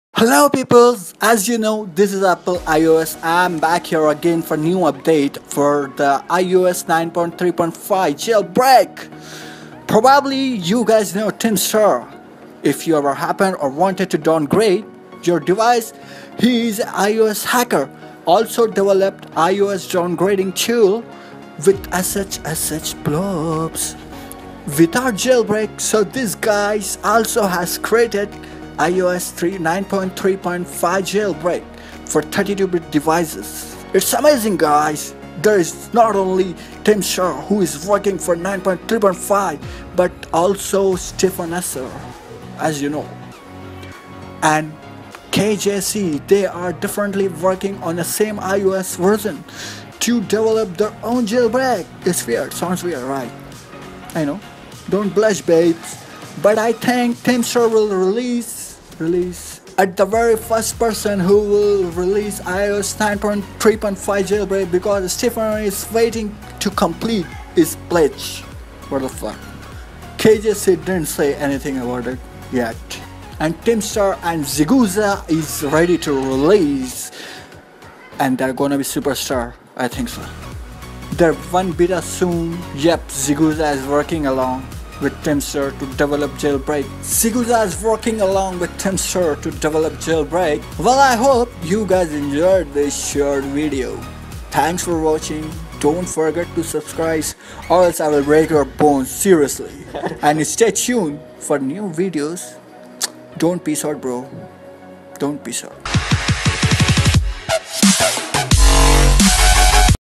IOS 9.3.5 JAILBREAK FOR 32 BIT BY TIHMSTAR & SIGUZA HAS BEEN CREATED!! RELEASING VERY SOON !!!! 😀
This video would be worth  if you are waiting to know something about 9.3.5 jailbreak for 32bit devices .